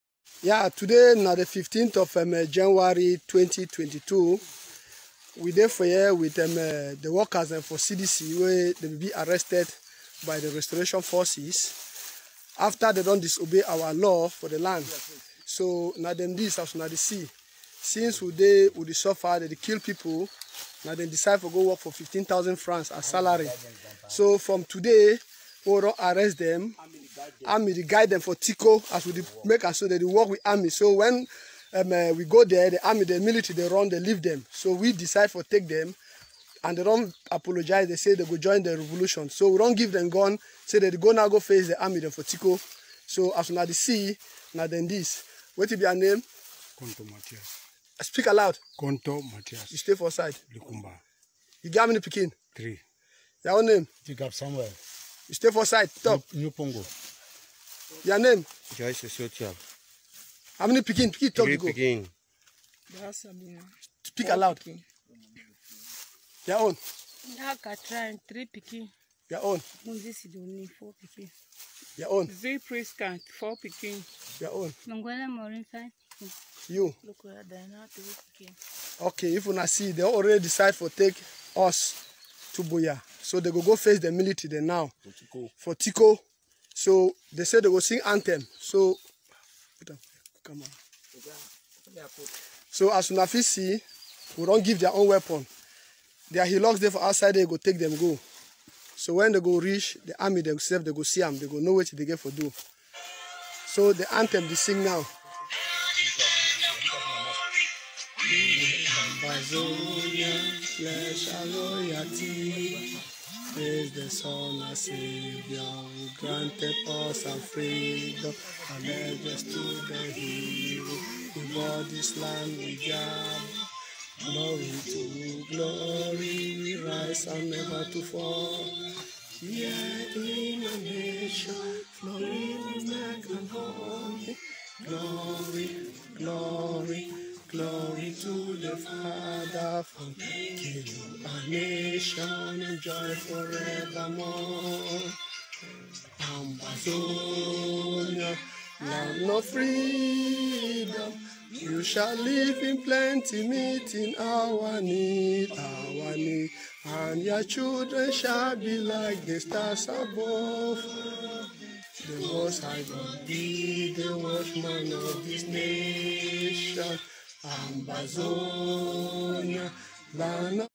Here are the CDC workers who were kidnapped by Ambaboys. Their names
Here are the CDC workers who were kidnapped by Ambaboys. Their names and numbers of children they have.